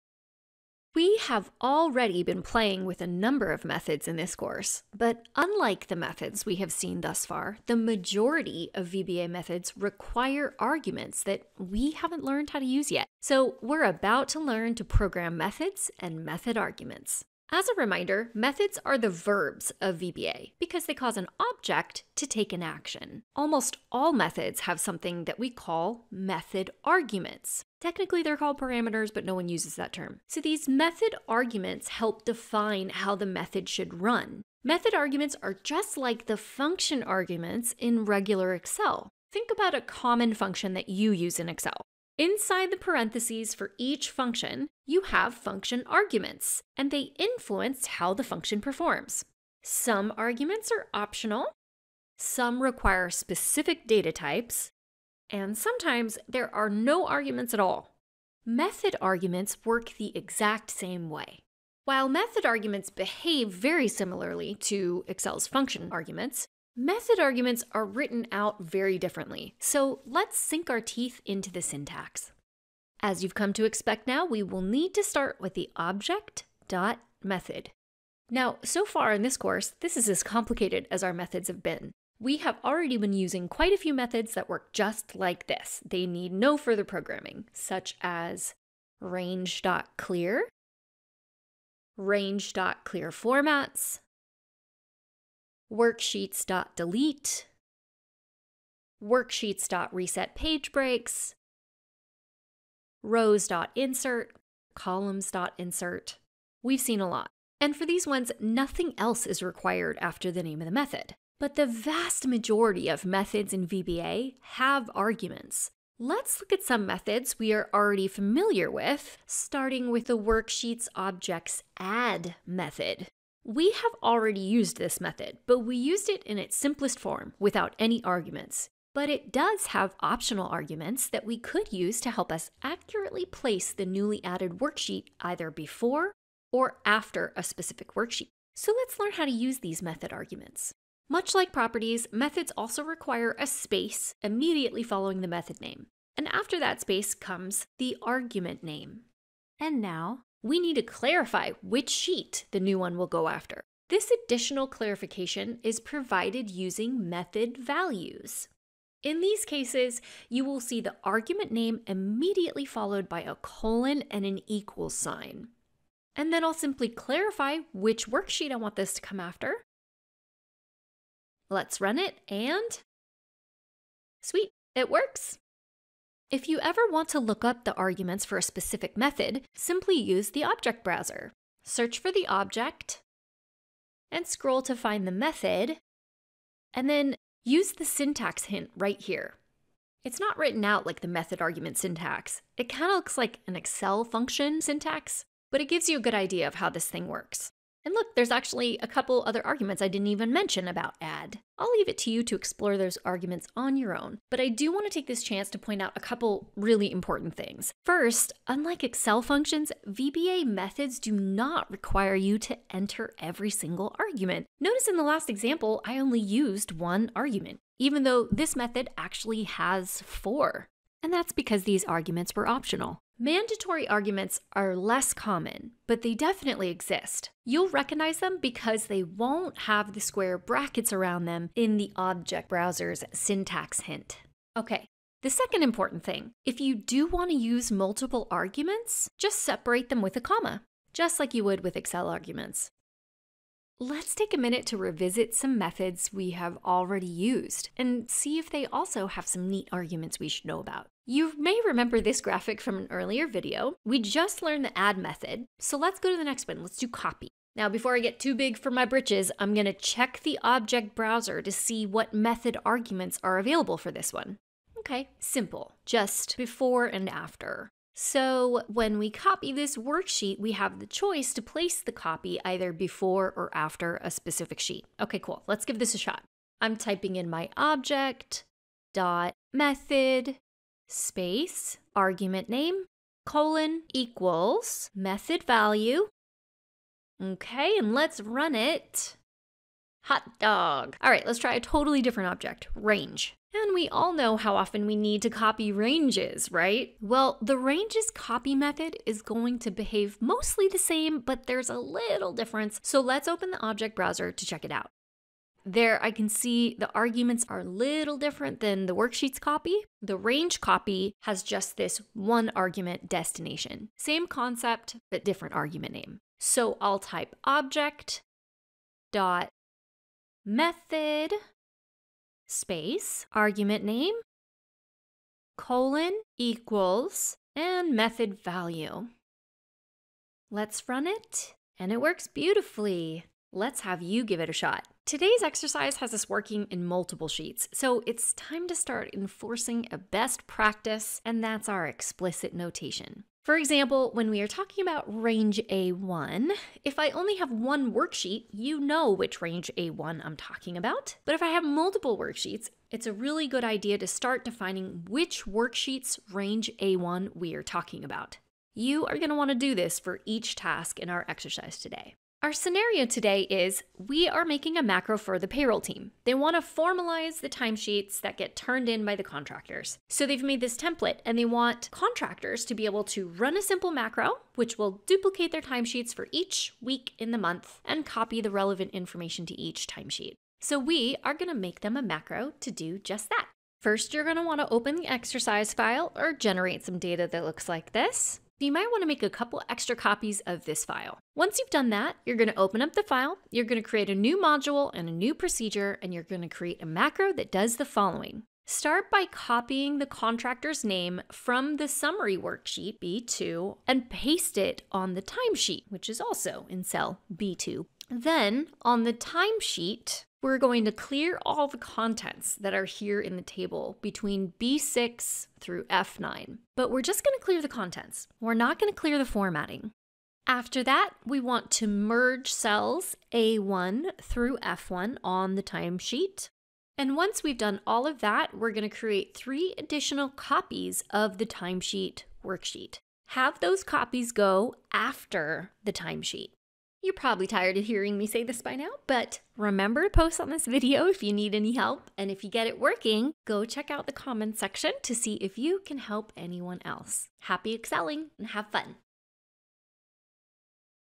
Methods and Method Arguments (Exercise)
VBA Fundamentals: Video 10

TOPICS
Start 0:00

CINTRODUCTION TO METHODS ARGUMENTS 0:18
This video reviews the basic methods already used in this course and takes the next step in method mastery: Method Arguments.

SYNTAX
Object.Method Argument:=Method Value
Basic Method Syntax 1:10
Method Argument Syntax 2:20

LOOKING UP METHODS & ARGUMENTS IN OBJECT BROWSER 2:58

EXAMPLES OF METHODS, METHOD ARGUMENTS, AND METHOD VALUES 4:12
 • Range.Clear 1:28
 • Range.ClearFormats 1:31
 • Worksheets.Delete 1:35
 • Worksheets.ResetPageBreaks 1:38
 • Rows.Insert 1:42
 • Columns.Insert 1:44
 • Worksheets.Add After:=Worksheets 2:09
 • Worksheets.Copy After:=Worksheets 4:12
 • Range.Copy Destination:=Range 4:50

REMINDER TO USE EXPLICIT PROGRAMMING STATEMENTS 5:33
 Worksheets.Range.Copy Destination:=Worksheets.Range

EXERCISE 6:02
 1. Copy the contractor name from the Summary worksheet (B2), and paste it on the Timesheet worksheet (B2).
 2. On the Timesheet worksheet, Clear Contents from B6 thru F9, without clearing the formatting.
 3. Merge cells A1:F1 on the Timesheet worksheet.
Create 3 additional copies of the Timesheet worksheet. Have the copies go after the Timesheet worksheet.